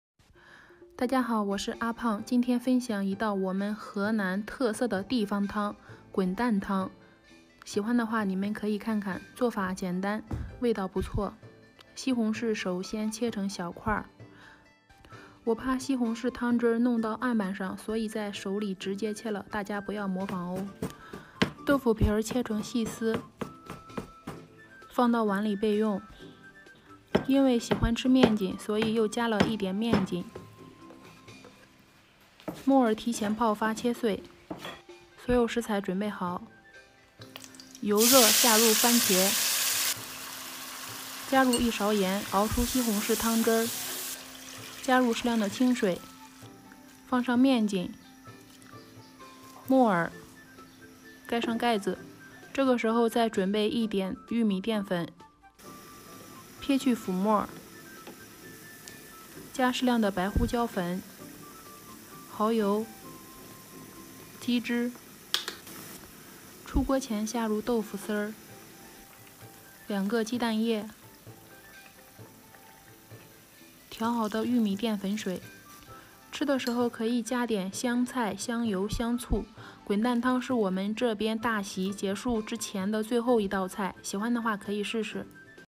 滚蛋汤这样做可以喝1年，什么蔬菜都能加，做法简单，营养美味【阿胖面食】
滚蛋汤这样做可以喝1年，什么蔬菜都能加，做法简单，营养美味